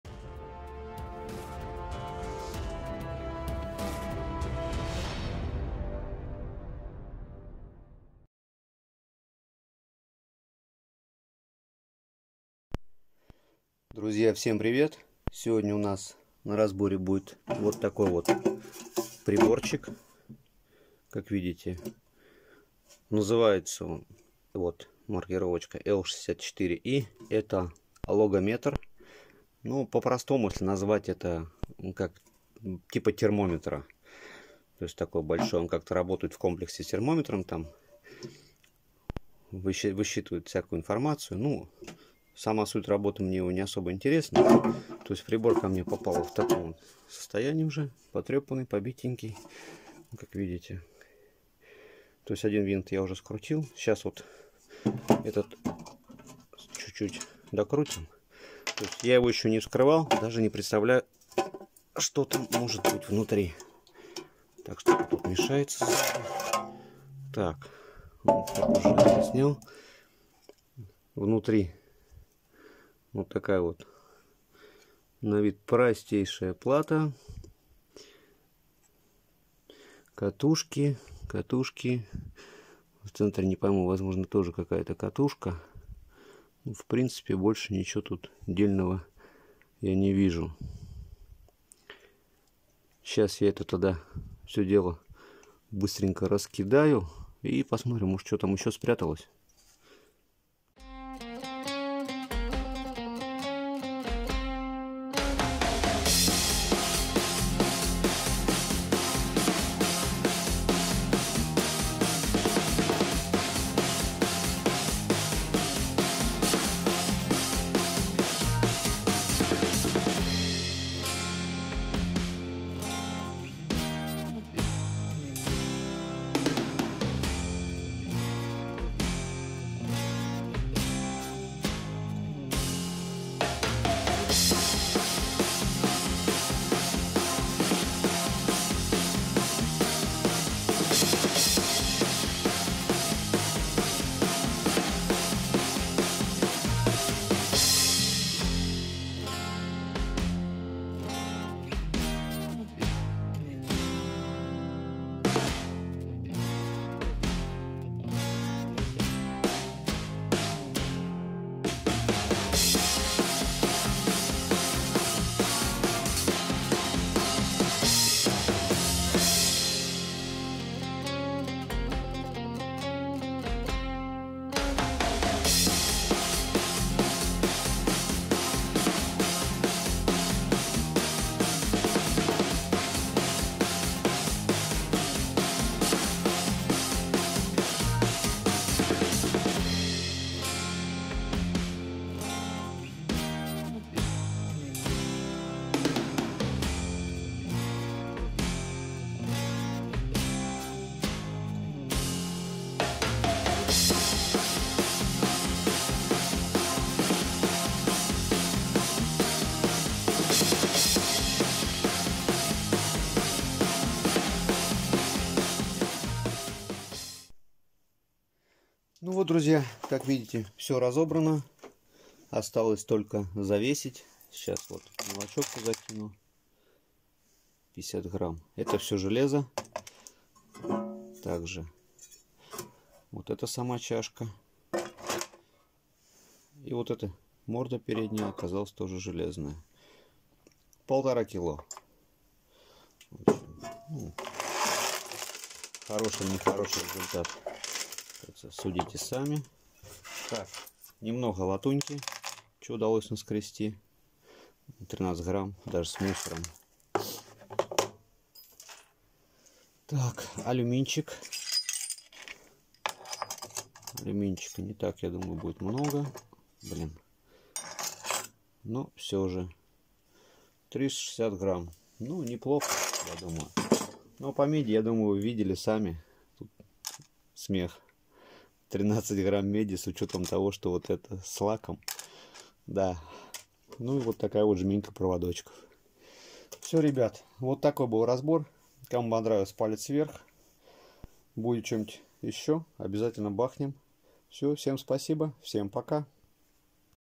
Логометр. Разбор в металлолом.
Всем привет. Сегодня будем разбирать советский девайс под названием логометр. Посмотрим сколько железа и цветных металлов в этом агрегате.